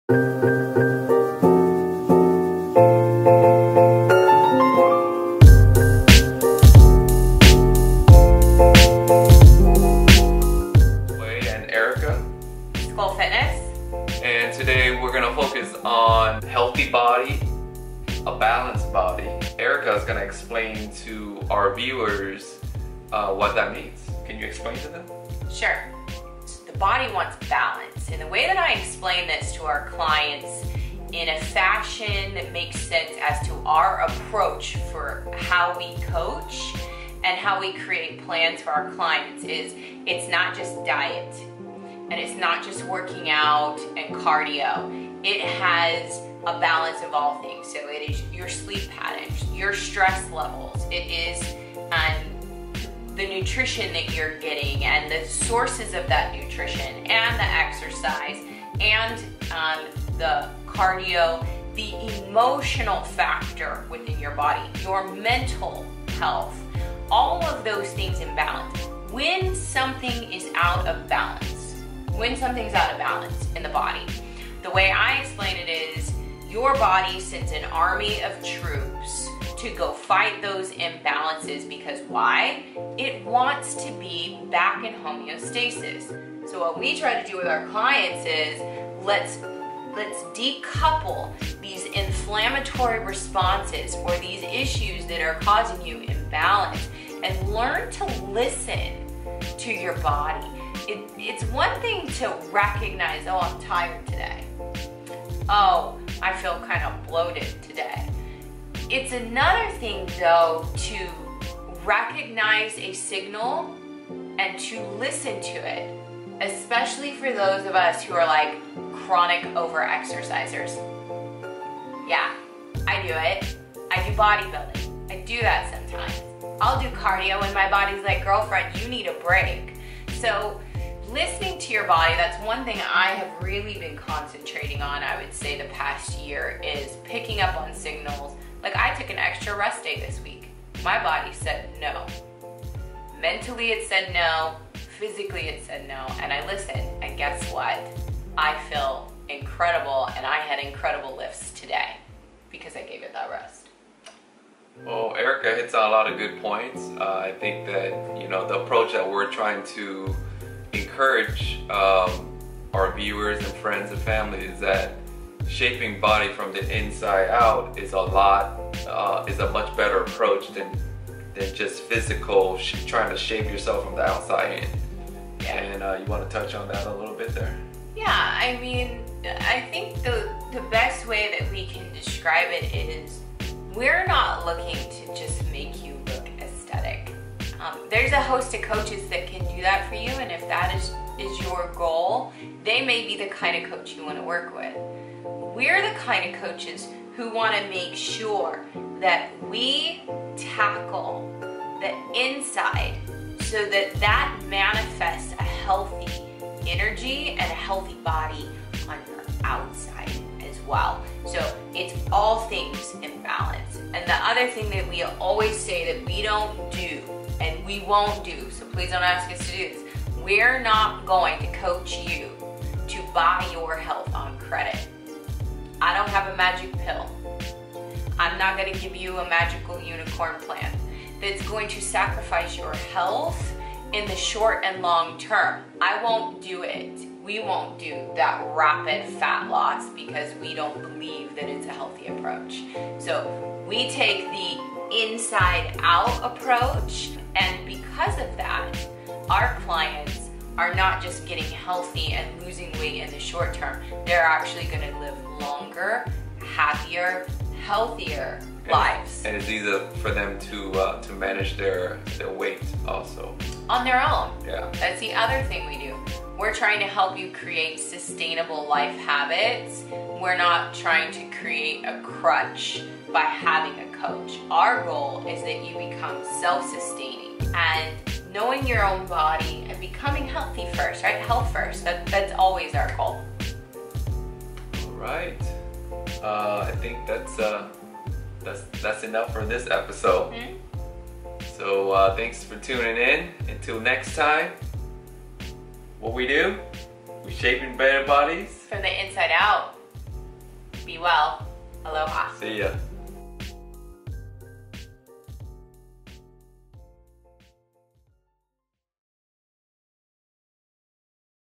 Wiki Wiki Wellness Tips with Sculpt Fitness
Coaches Erica and Tway sit down for a few minutes with you to chat about the Sculpt Fitness approach to health and wellness!  Our information is below and we'd love to hear from you.

Mahalo Nui for Tuning In,
Erica & Tway

Coach "E" Erica: @bulletproofbikini
Coach Tway: @twaysian